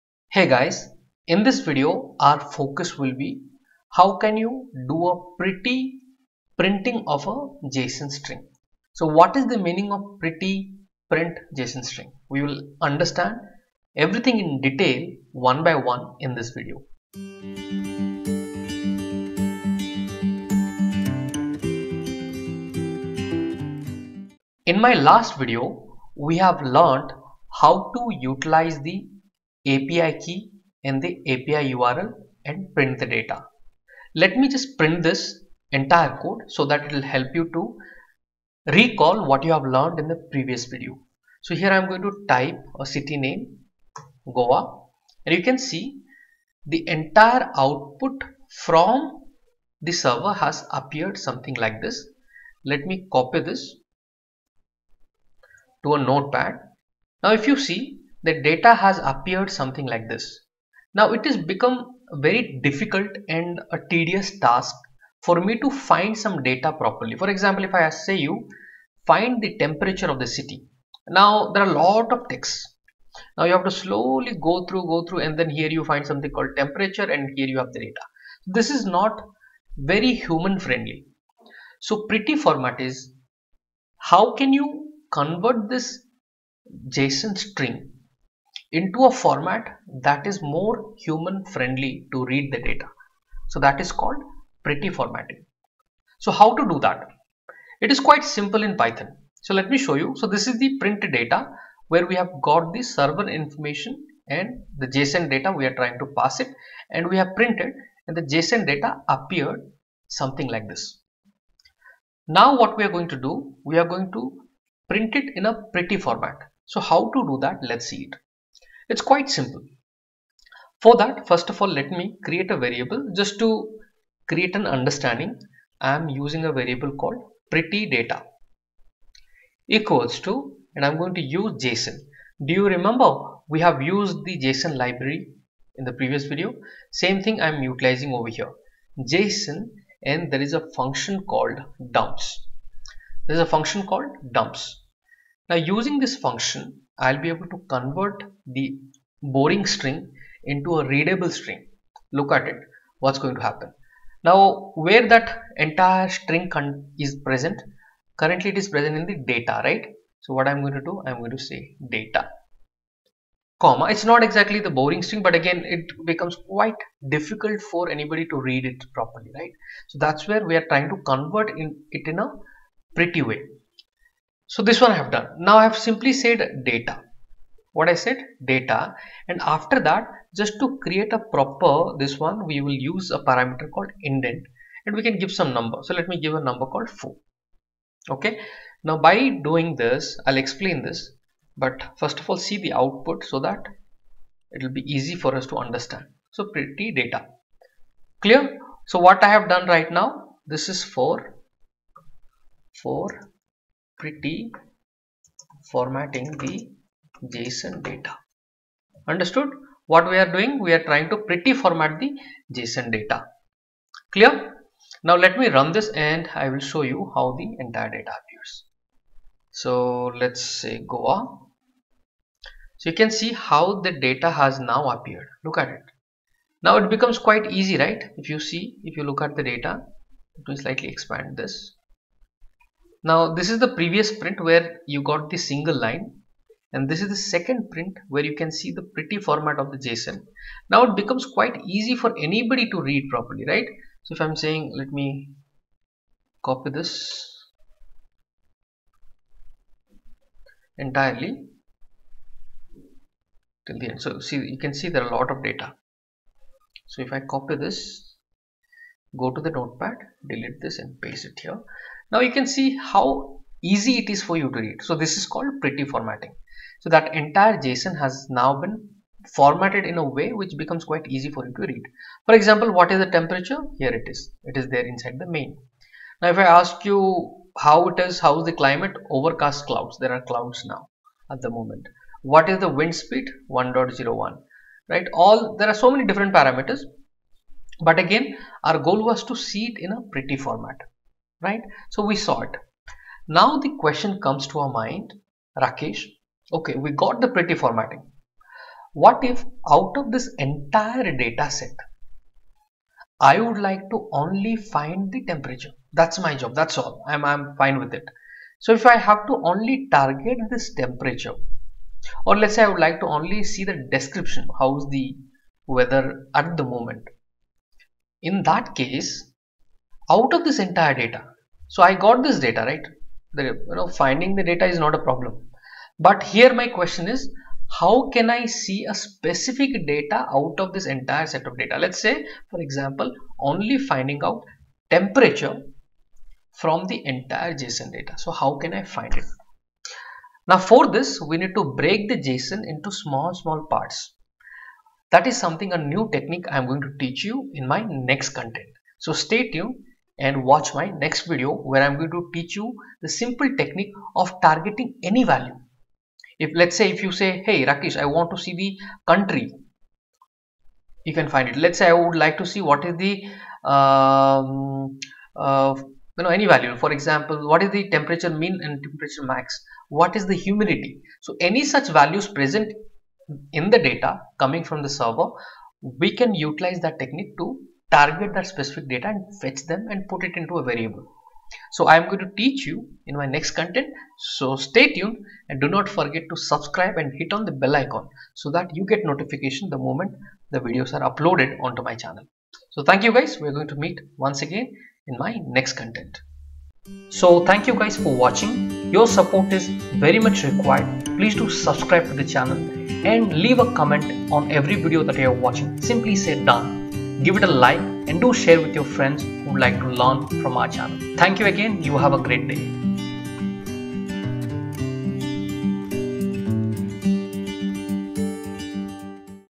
Python Pretty Print JSON String: Enhance Readability with Proper Formatting of JSON
Learn the art of enhancing your JSON data's readability using Python's pretty print technique. In this tutorial, discover how to transform complex JSON strings into well-structured and organized output. Dive into the world of indentations, line breaks, and proper spacing to make your JSON data more human-friendly. Whether you're working with APIs, data manipulation, or configuration files, mastering pretty printing will streamline your coding experience. Elevate your Python skills and create cleaner, more manageable JSON representations. Watch now and bring order to your JSON data!

In this video get answers to:
How do you pretty print a JSON string in Pythpn?
How to print object to JSON string in Python?